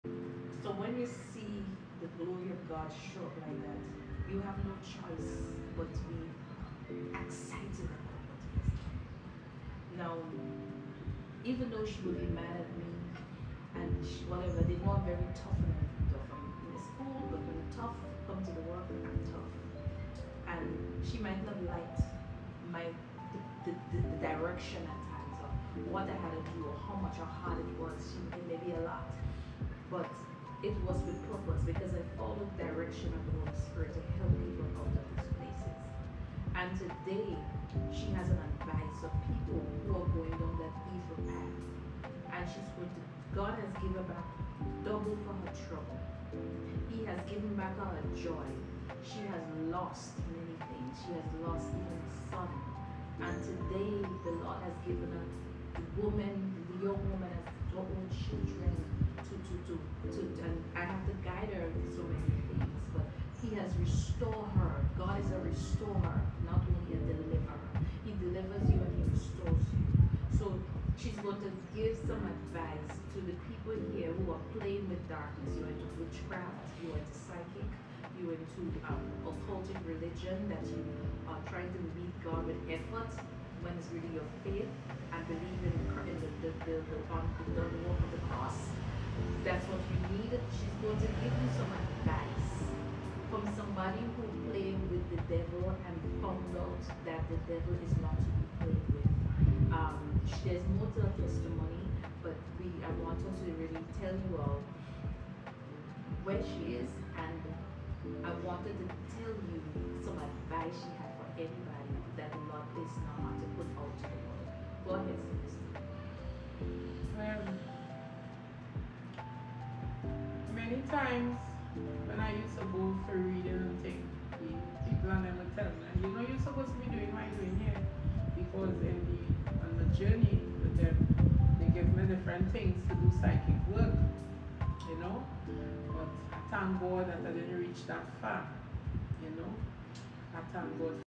Spiritual advise on deliverance journey
We need to understand the spiritual world to be free learning how to defeat our enemies